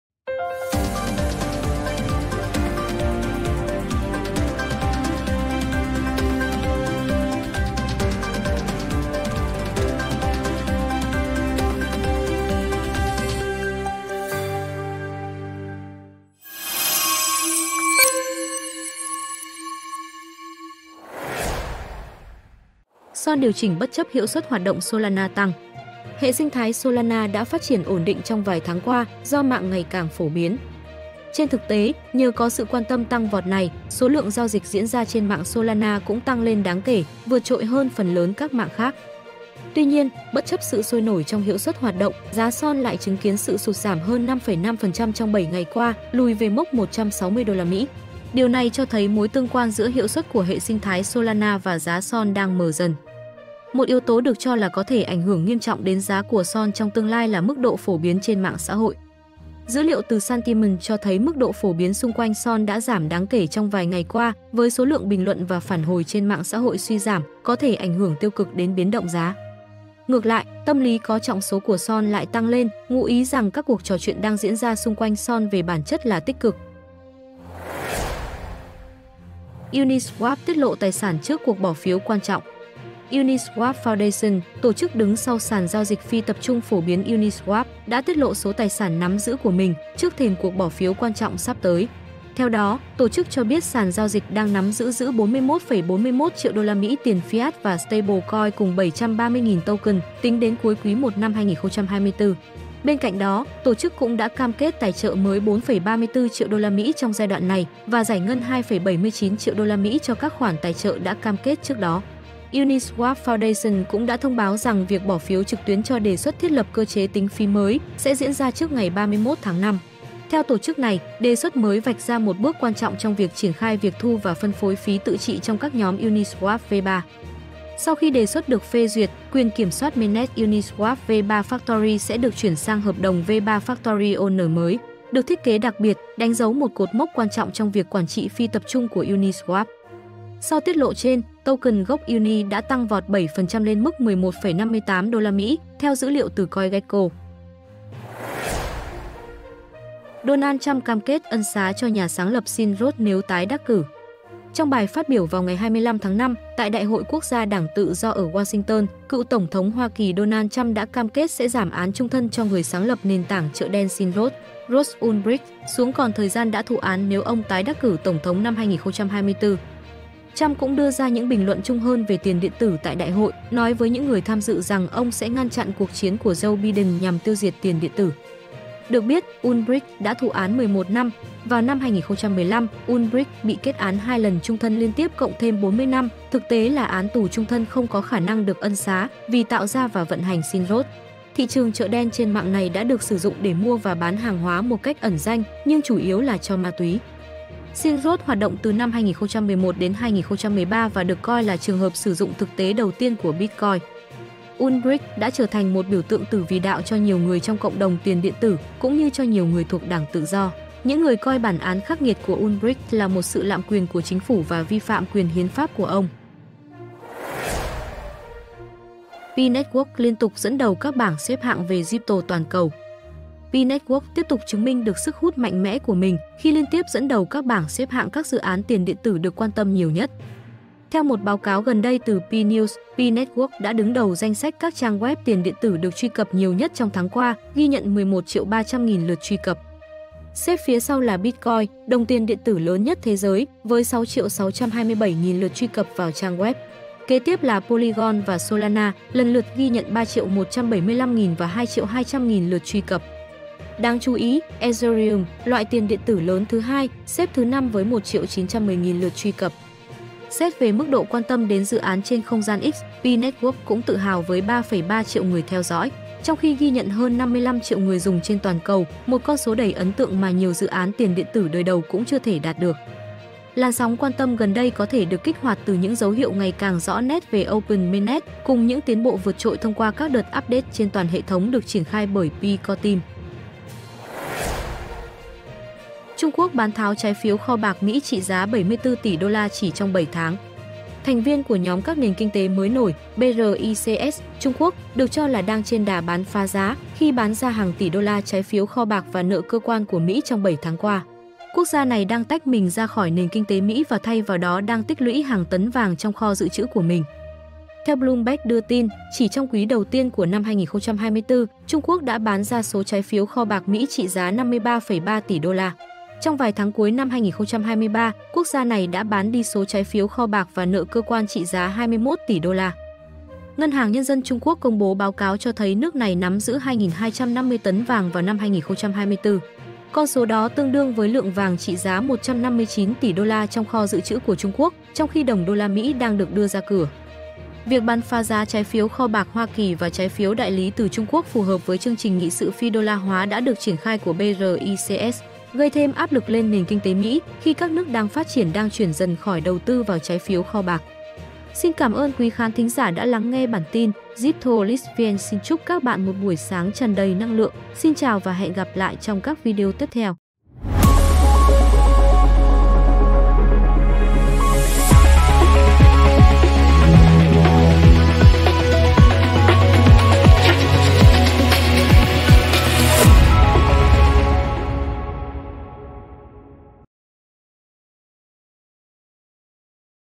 [5 Phút Crypto] - Pi Network Liên Tục Dẫn Đầu Các BXH Về Crypto Toàn Cầu
Mời các bạn cùng xem các tin tức crypto nổi bật trong buổi sáng ngày hôm nay:

+ 00:23 - SOL điều chỉnh bất chấp hiệu suất hoạt động Solana tăng
+ 01:19 - Uniswap tiết lộ tài sản trước cuộc bỏ phiếu quan trọng
+ 02:30 - Donald Trump cam kết "ân xá" cho nhà sáng lập Silk Road nếu tái đắc cử
+ 03:39 - Pi Network liên tục dẫn đầu các BXH về Crypto toàn cầu
+ 04:46 - Trung Quốc "bán tháo" trái phiếu kho bạc Mỹ trị giá 74 tỷ đô la chỉ trong 7 tháng

🌟🚀 Link mời đăng ký tạo tài khoản trên Ice Network Cryptoleakvn